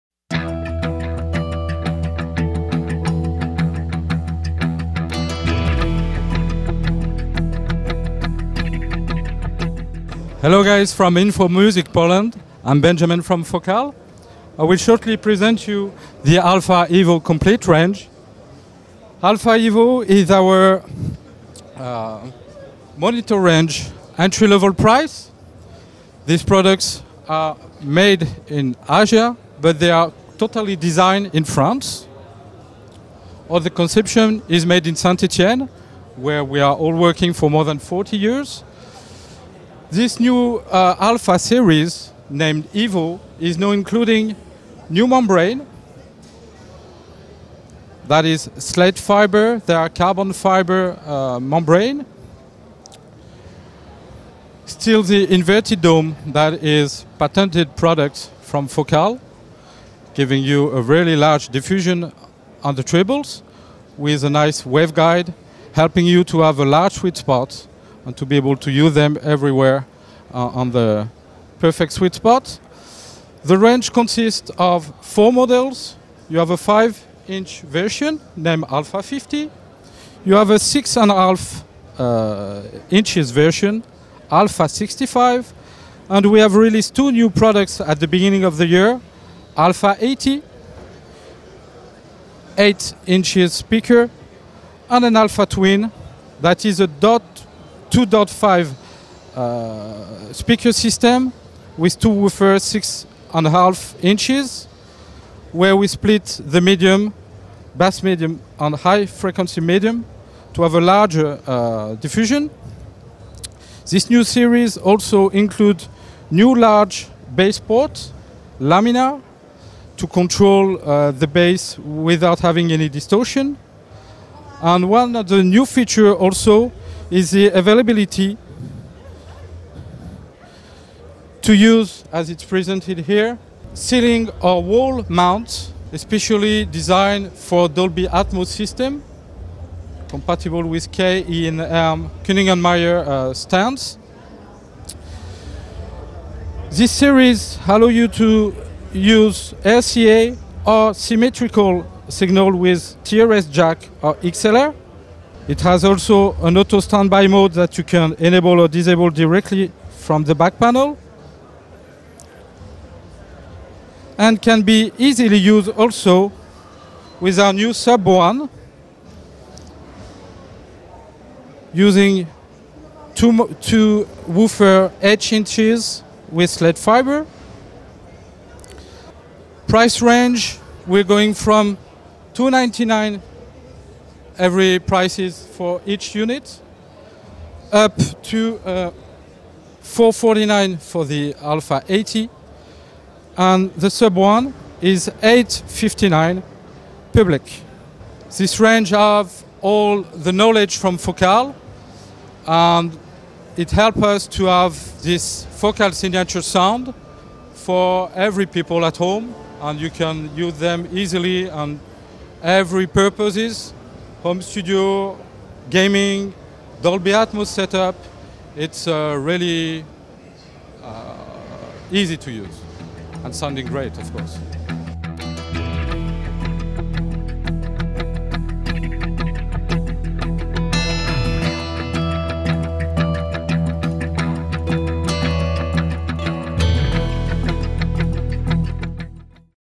Focal ALPHA EVO - studio monitor [ISE'22]
Focal to jeden z wiodących producentów profesjonalnych monitorów studyjnych, które cieszą się wysokim uznaniem wśród realizatorów dźwięku. Jednak nawet rynkowy lider oprócz konstruowania nowych modeli urządzeń odświeża i unowocześnia te już istniejące.

w skrócie:
Monitor Alpha 80 Evo, który po liftingu otrzymał:
wejście typu jack TRS
funkcja automatycznego przejścia w tryb czuwania
uchwyty, dzięki którym da się je mocować na ścianach i suficie
cena: 549 USD

Focal Alpha Twin Evo to:
monitor z dwoma wooferami 6.5 cala i takim samym, jak w Alpha 80 Evo
zastosowano dwa głośniki niskotonowe zamiast jednego większego
cena: 659 USD

Focal Sub One
pracują w nim dwa ustawione przeciwsobnie 8-calowe przetworniki basowe
Wbudowany wzmacniacz dostarcza moc 200 W RMS
Pasmo 45-120 Hz, regulacja fazy (0-180 stopni), mozliwość odwracania polaryzacji
cena: 999 USD
00:00 Focal Alpha EVO
00:19 Seria monitorów studyjnych Alpha EVO
01:20 Focal Alpha 65 EVO
01:30 Focal Alpha 80 EVO
01:44 Focal Alpha Twin EVO
03:00 Subwoofer Focal